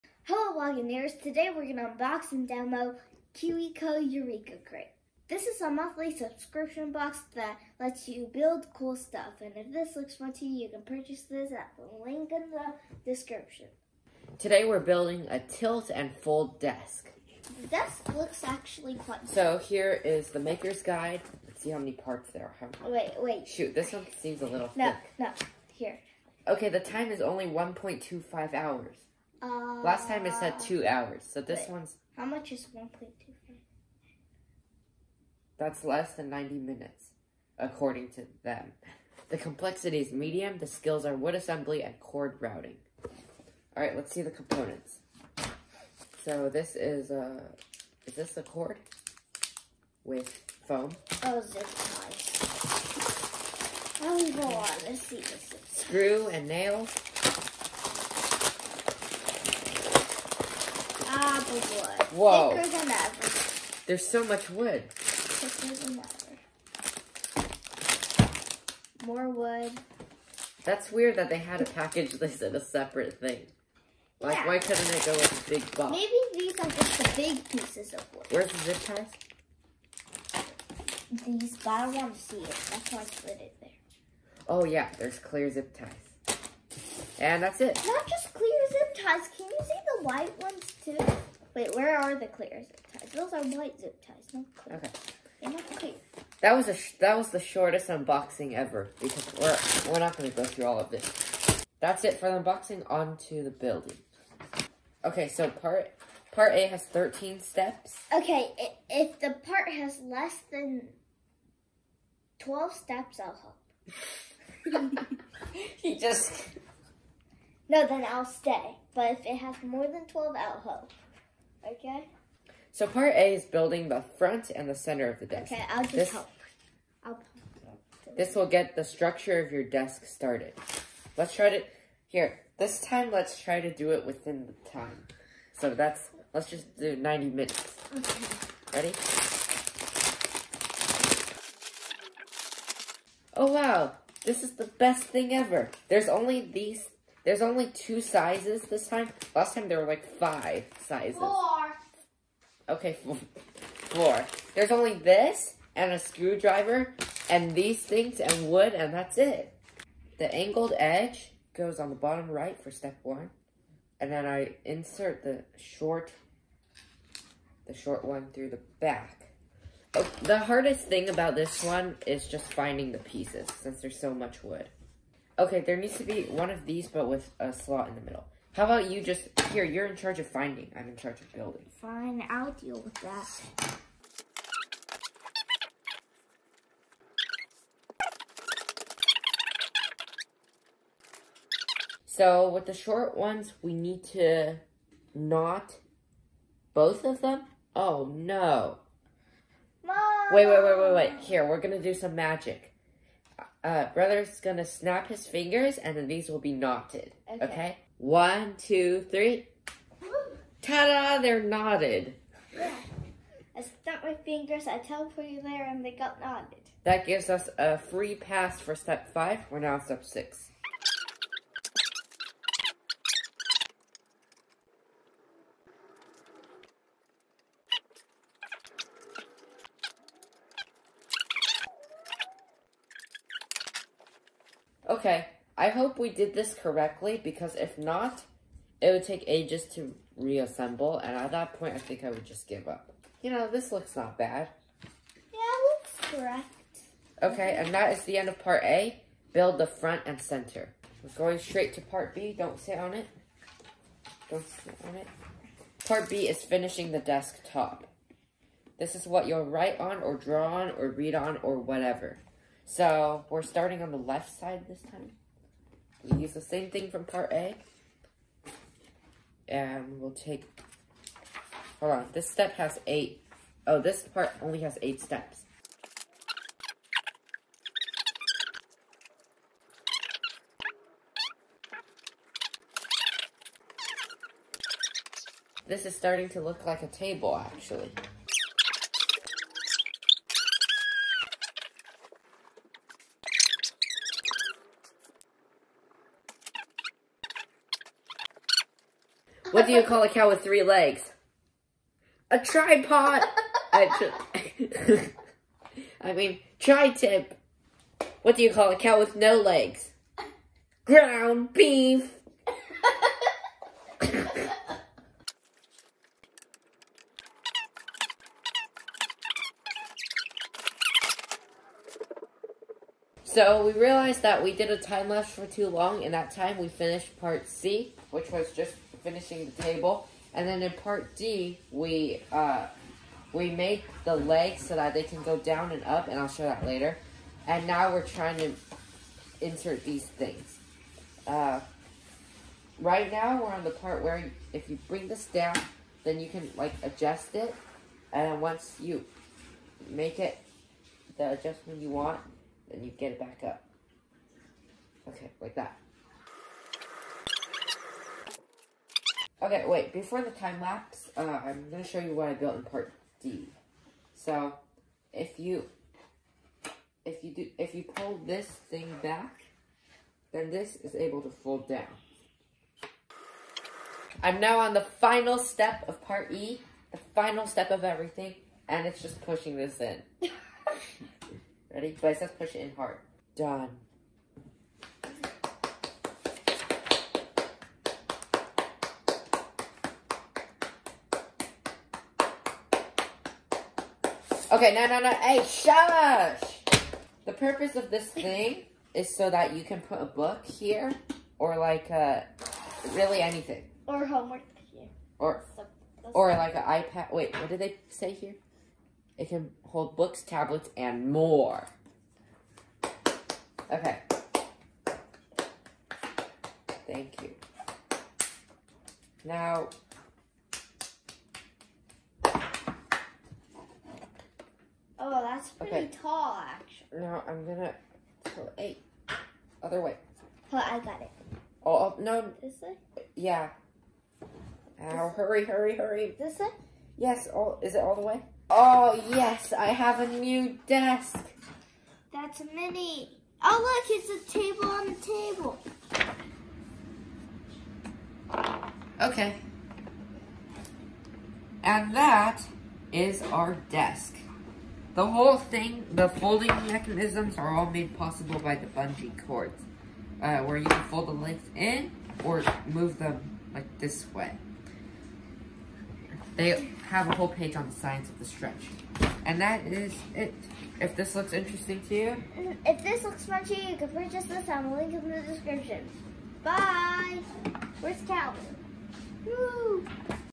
We Built a Tilt-and-Fold Desk! KiwiCo Eureka Crate Unboxing and Review 2022 | STEM Project
We unbox, demo, and review the Eureka Crate from KiwiCo. In this month's subscription box, we will be building a tilt-and-fold desk.